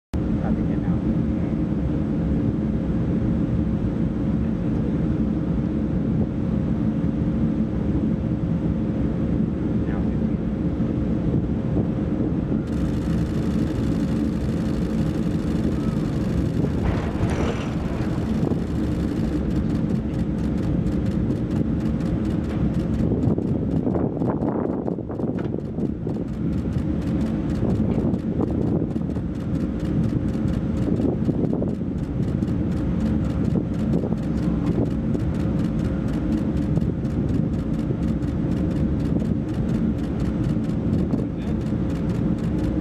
USS Gerald R. Ford (CVN 78) Full Ship Shock Trials - Event 1
Full Ship Shock Trials - Event 1
UNITED STATES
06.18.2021

The aircraft carrier USS Gerald R. Ford (CVN 78) completes the first scheduled explosive event of Full Ship Shock Trials while underway in the Atlantic Ocean, June 18, 2021. The U.S. Navy conducts shock trials of new ship designs using live explosives to confirm that our warships can continue to meet demanding mission requirements under harsh conditions they might encounter in battle.

Film Credits: Video by Petty Officer 2nd Class Ryan Seelbach

Warship78
This Is Ford Class
Full Ship Shock Trials